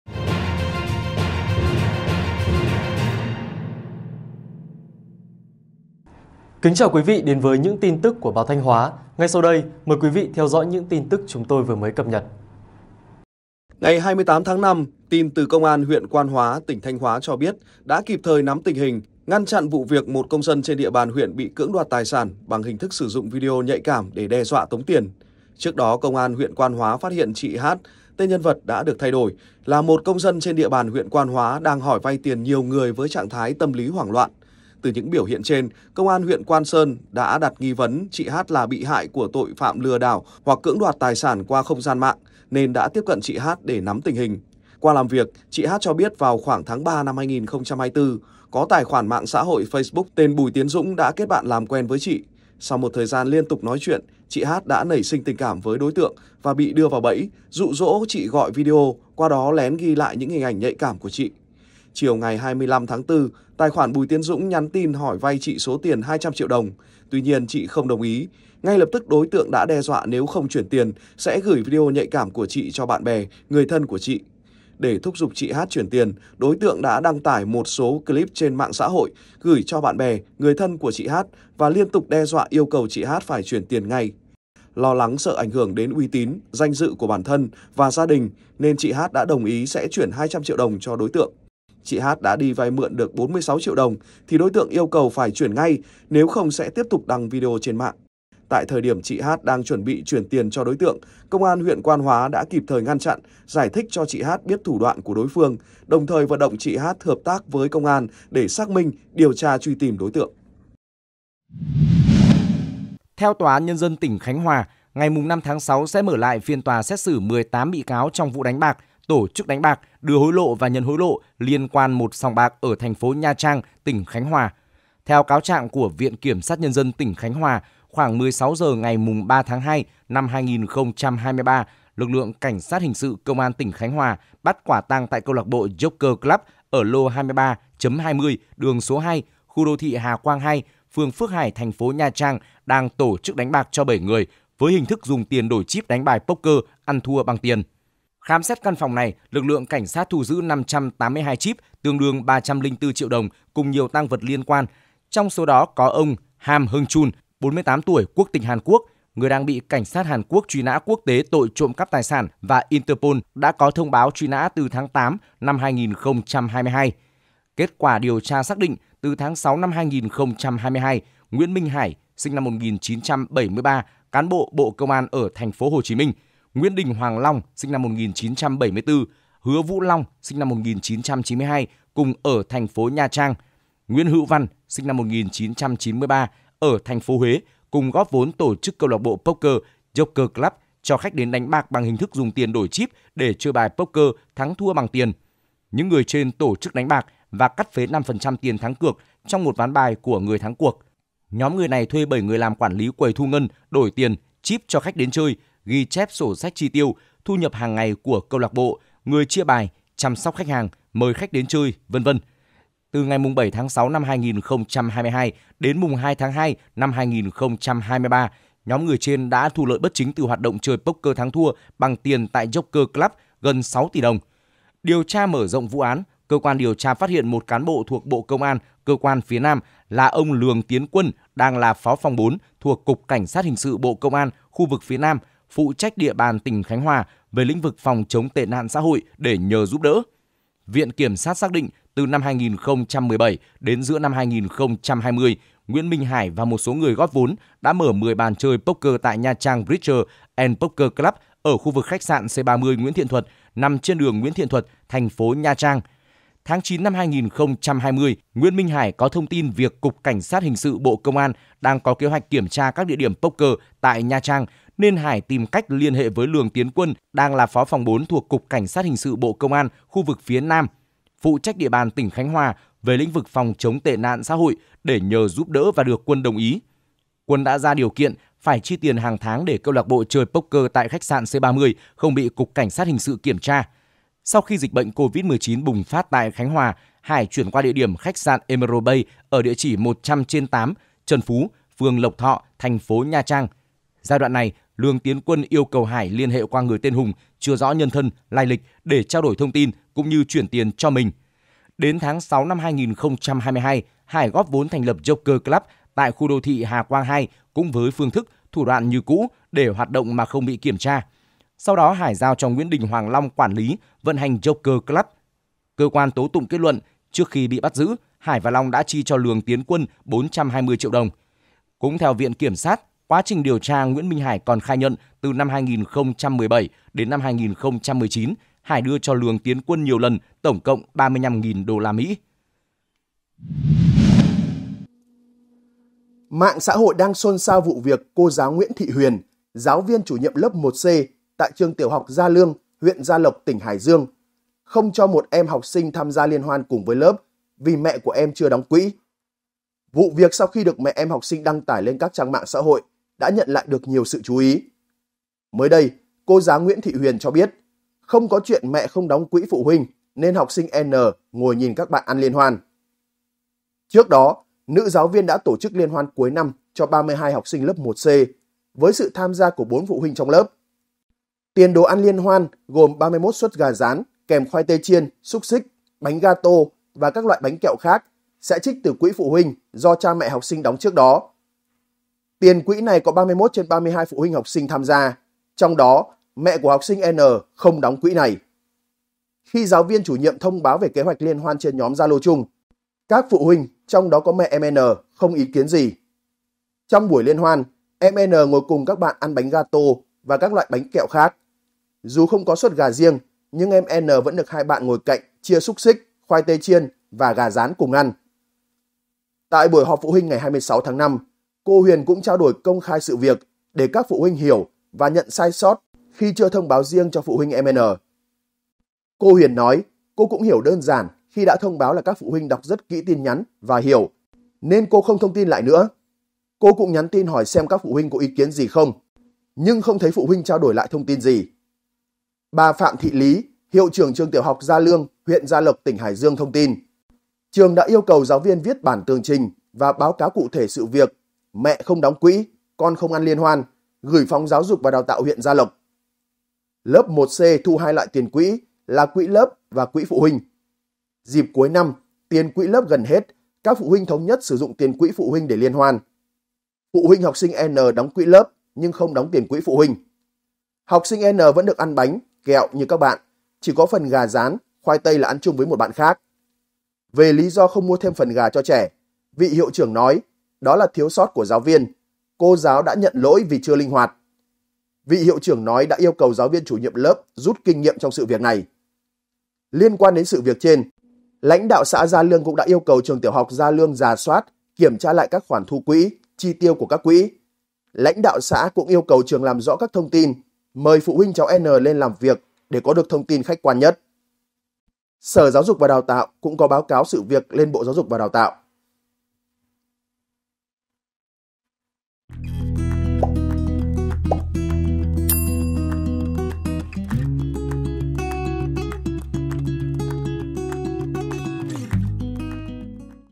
Gã trai lén quay lại cảnh nhạy cảm để tống tiền bạn gái
Tin nóng | Gã trai lén quay lại cảnh nhạy cảm để tống tiền bạn gái
Quá trình quen biết qua mạng xã hội Facebook, một cô gái ở Thanh Hóa đã bị một người đàn ông dụ dỗ gọi nói chuyện bằng video, sau đó lén quay lại cảnh nhạy cảm để tống tiền